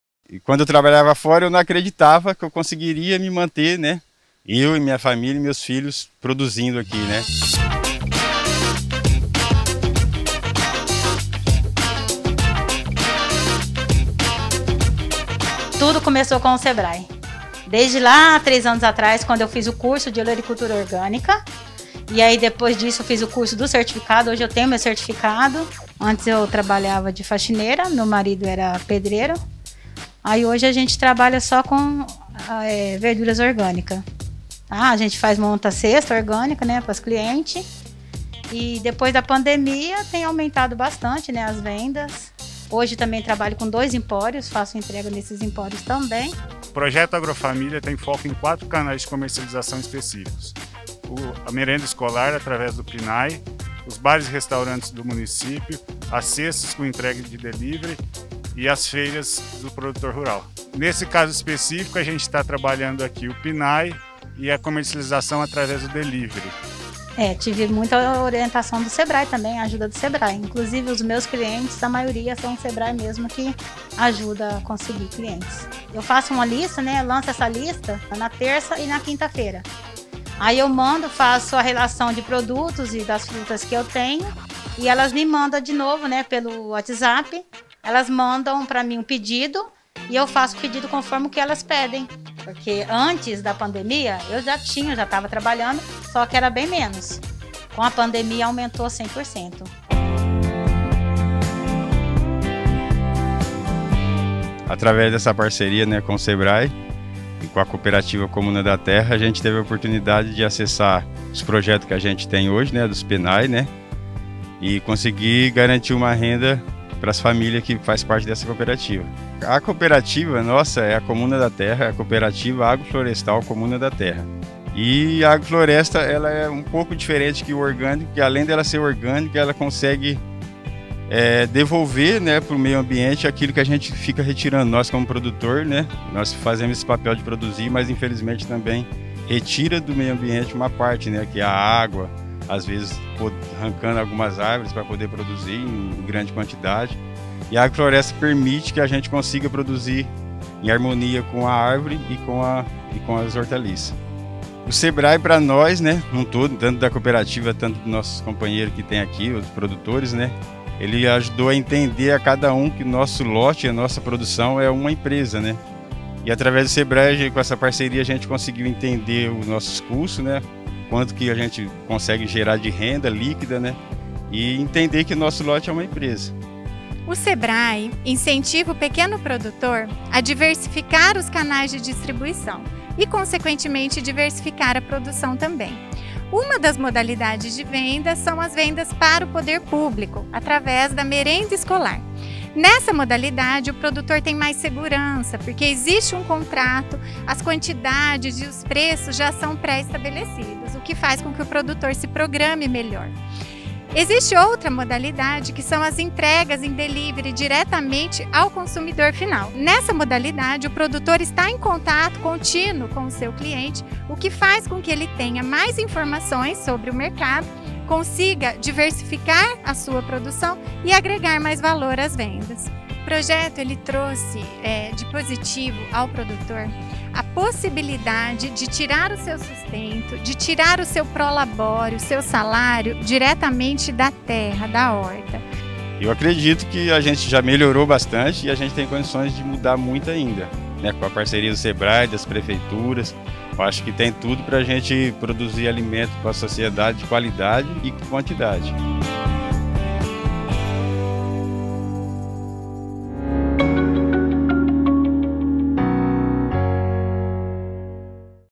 Parcerias que dão certo - Projeto Agro Família
O projeto Agro família organizou os agricultores familiares para se aproximar do mercado institucional e consumidor final.